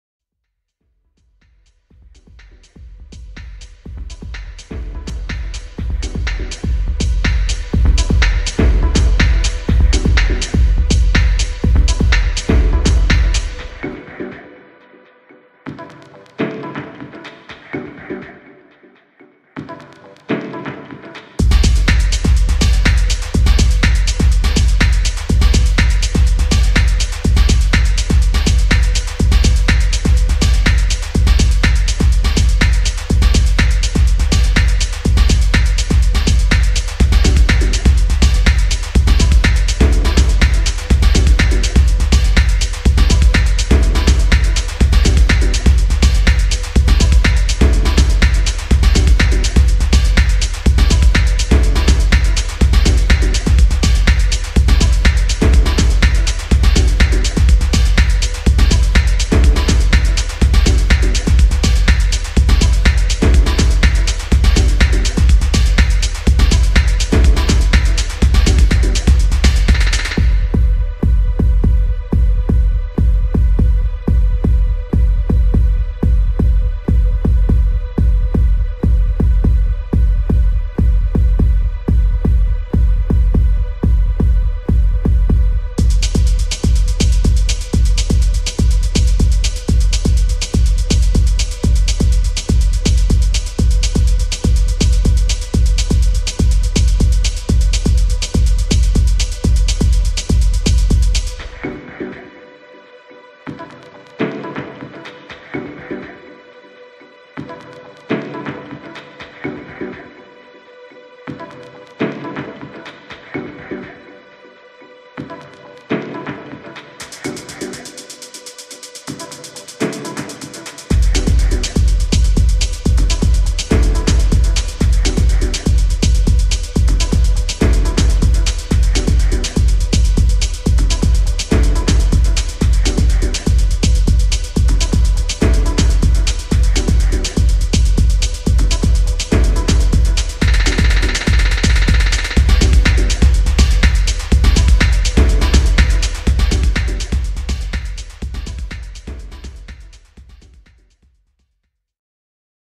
Sound_District003_Julien_Bracht HD
The official after-movie of Sound District Promotions - SD003 party with Julien Bracht (Cocoon, GE) and Robert Drewek (Rawax, GE). What an incredible night with such great talent and amazing people.